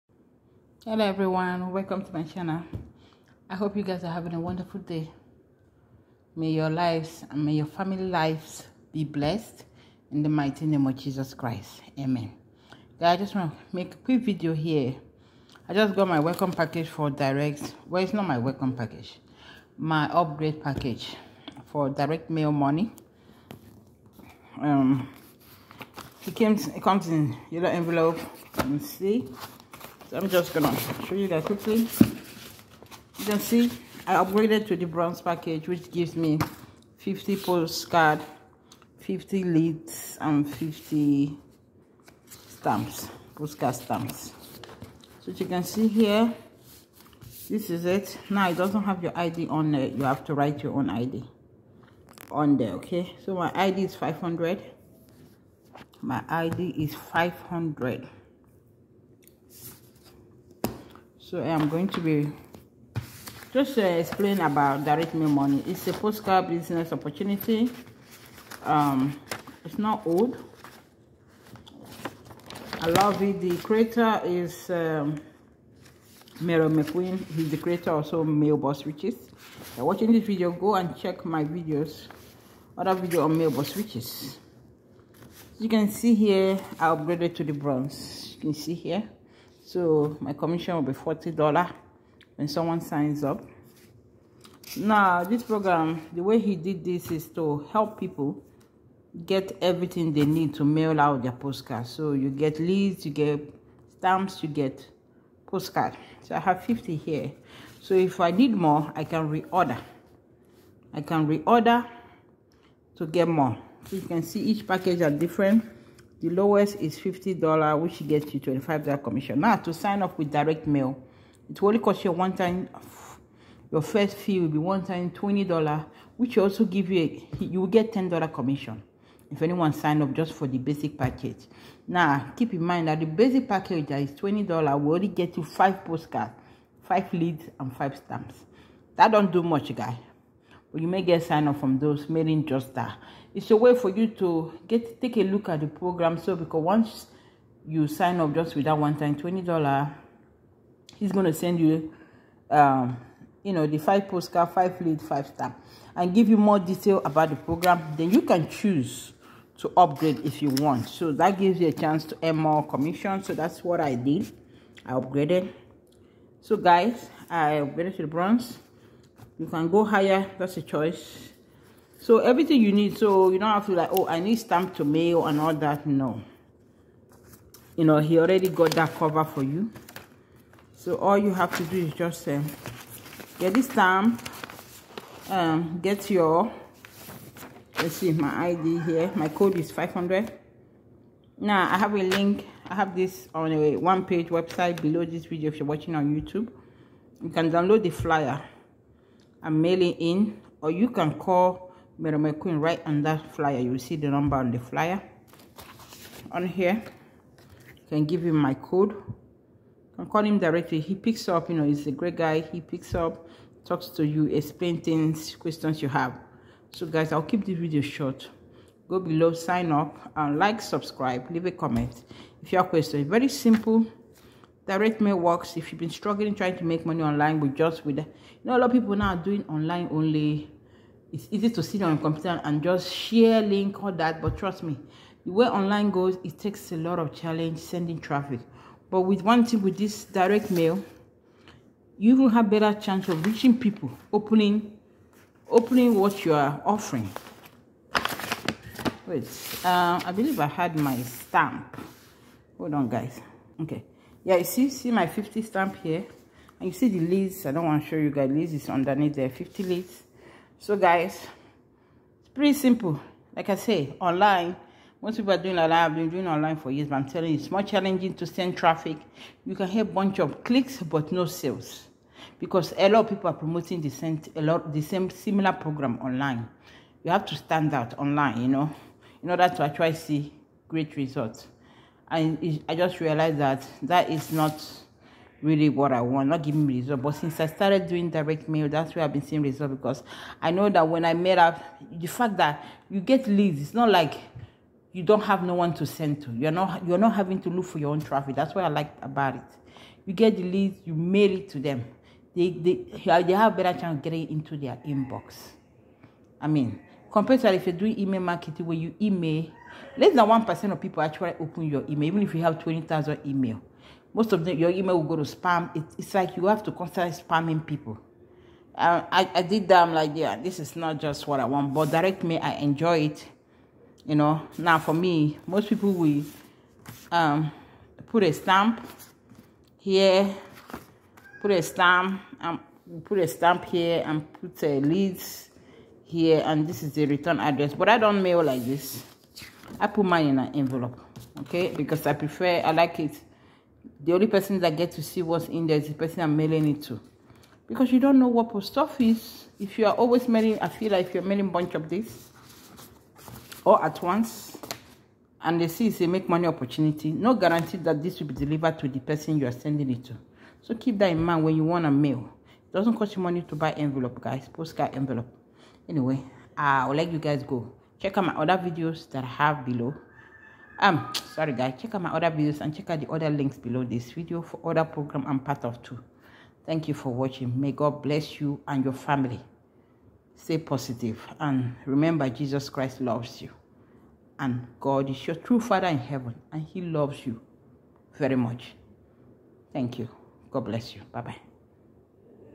Direct Mail Money Upgrade Package Explained - Make Money Mailing Postcards From Home
Direct Mail Money
Make Money From Home Mailing Postcard!
All In One Advertising Income Package
Receive Cash Weekly
Earn $10 To $3500 In Cash Commissions Per Order
The entry Fee is just $20 and you receive 5 color postcards, 5 leads(opportunity Seekers) and 5 stamps.

Use Code when you call admin. Code: 500

Consider offline make money opportunities
Phase 10 program

Do you need Webhosting?

What is direct mail?